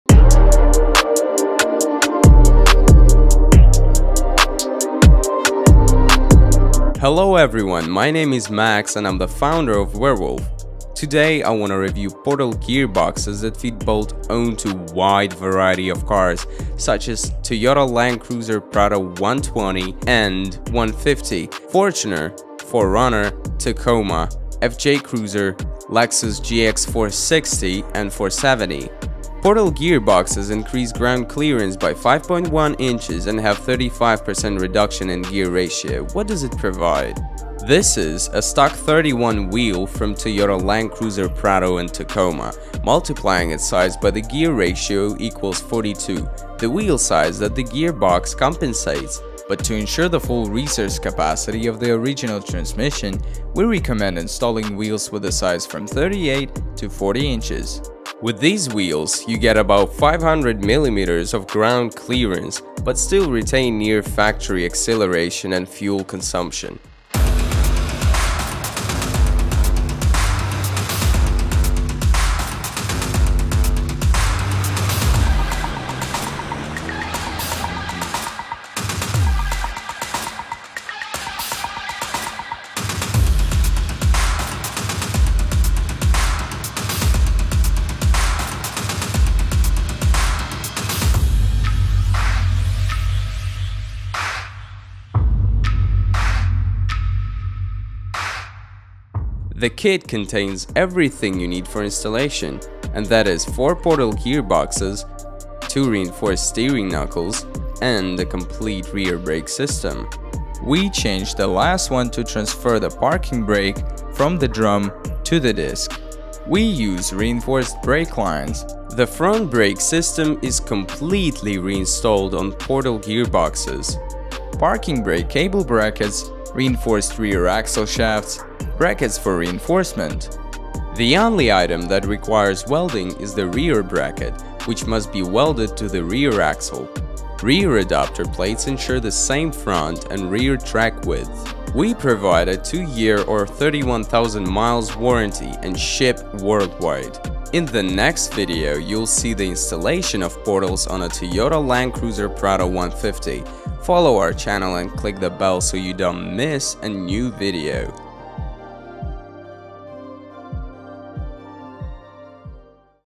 Review of bolt-on portal gearboxes kit for Toyota SUVs with independent front suspension(in English)
Models which the kit fits bolt-on for:
Tacoma, Hilux, 4Runner, Fortuner, FJ Cruiser, Prado 120/150 and Lexus GX

What are the main characteristics of portal gearboxes?
And what do they give in simple terms?
Which wheels do we recommend and why?
What does the kit contain and why?